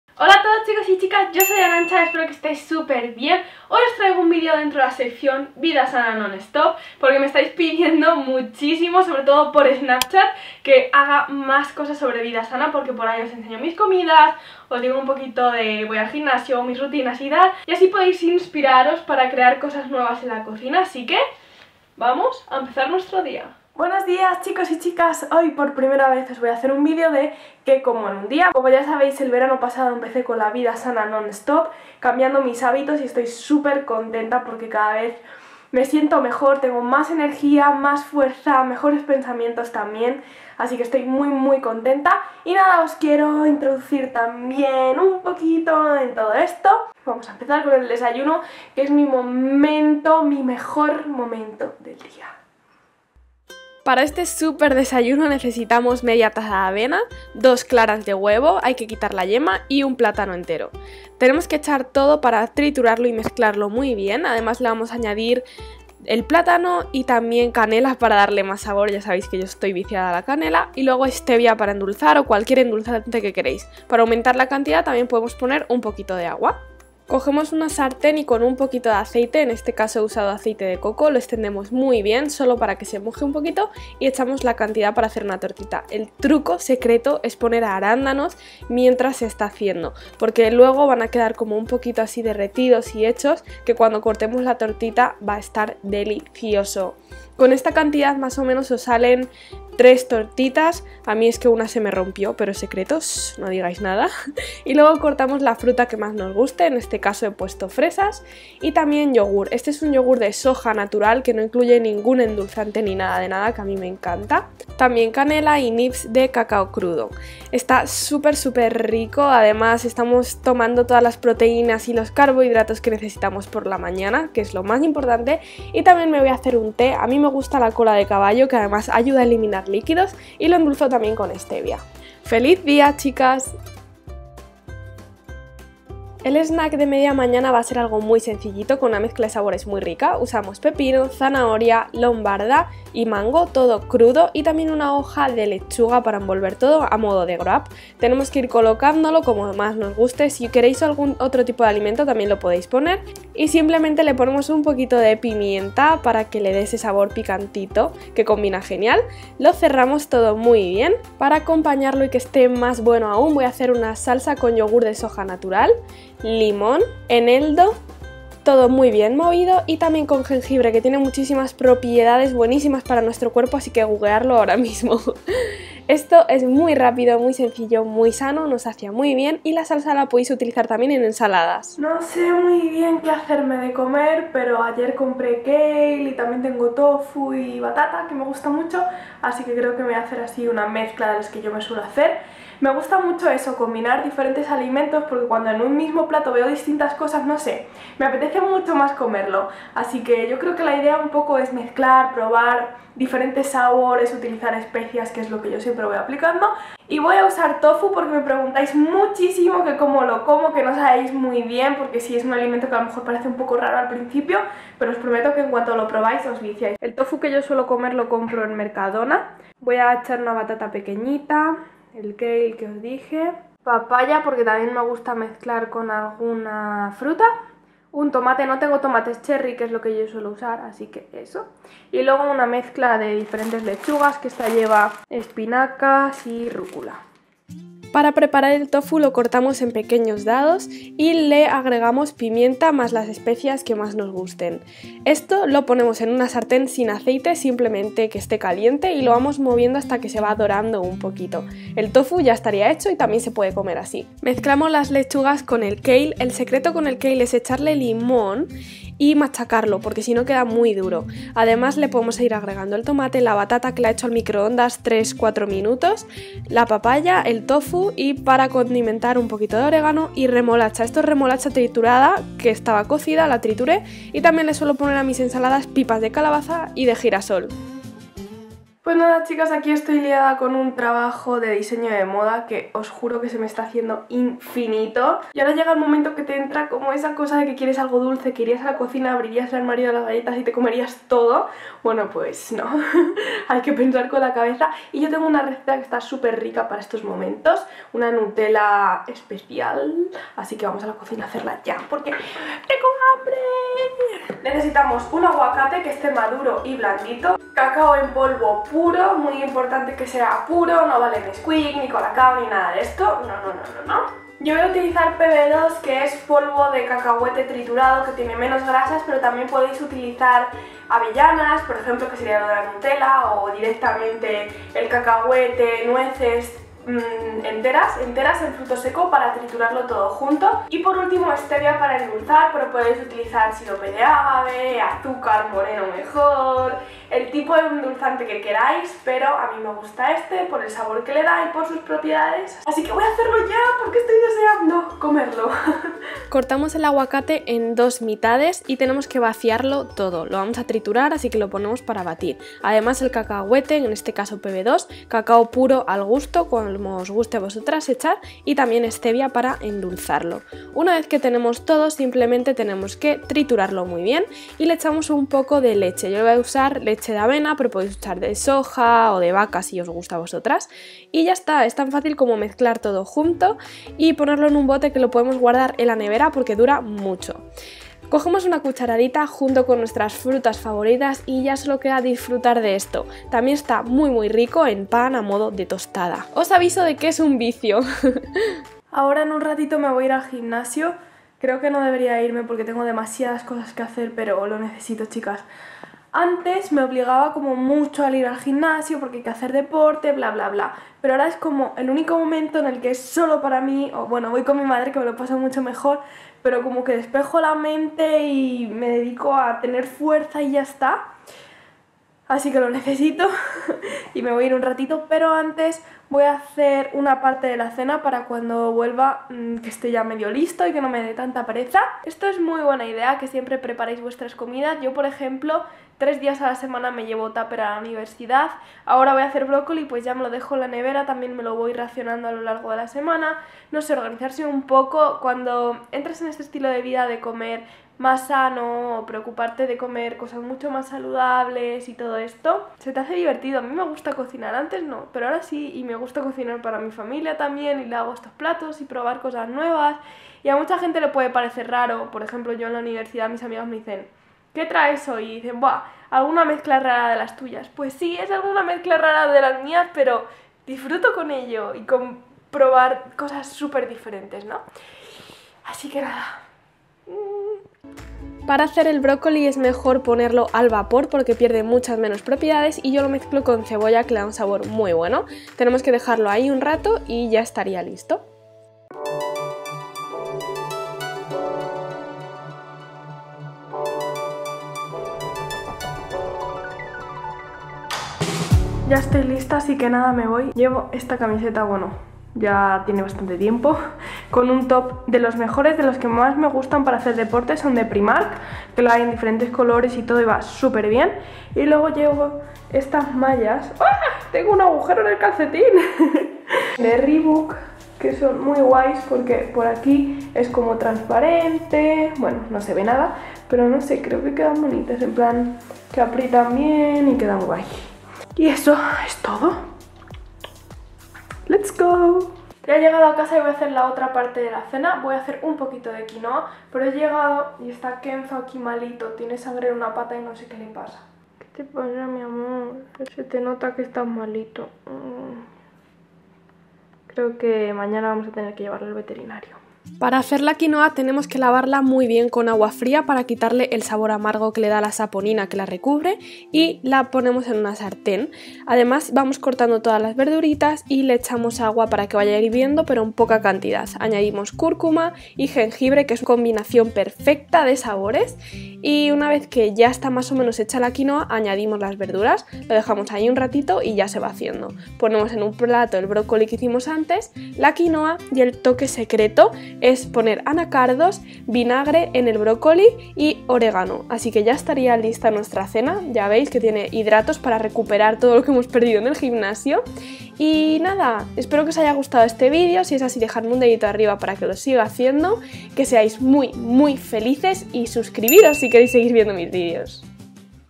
¿Qué como en un día? 5 RECETAS SANAS Y RICAS | Nightnonstop
¡Hoy me acompañáis en mi rutina de día! Así os enseño mis comidas con recetas sanas, ricas y vegetarianas. Si todavía no os habéis unido, es el momento de comenzar la vidasananonstop :D

● Información
Editor: Final Cut Pro X
Cámara: Nikon D5100
Objetivo: 50 mm f/1,8

Todos los alimentos podéis encontrarlos en supermercados o herbolarios.
Kale: Hipercor
Tofu: Mercadona
Yogurt de soja: Hipercor y Carrefour
Quinoa: Ahorra Más y herbolarios
Cacao puro: Mercadona